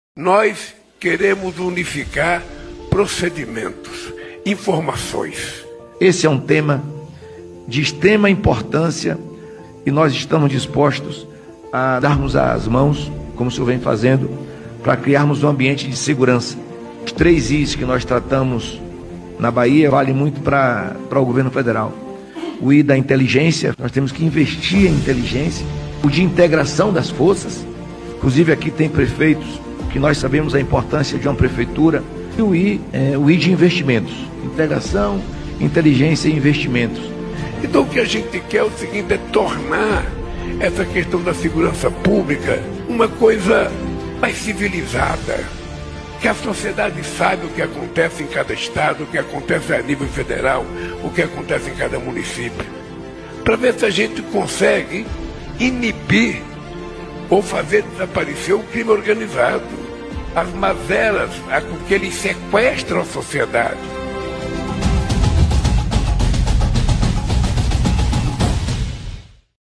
Presidente Lula: 'Precisamos de inteligência, de integração das forças e de investimentos'
"Precisamos de inteligência, de integração das forças e de investimentos" 🔴 Assista a fala do governador da Bahia, @jeronimorodriguesba , no debate sobre a PEC da Segurança Pública.

O Governo Lula pretende conferir status constitucional ao Sistema Único de Segurança Pública (Susp), instituído em 2018, por lei ordinária.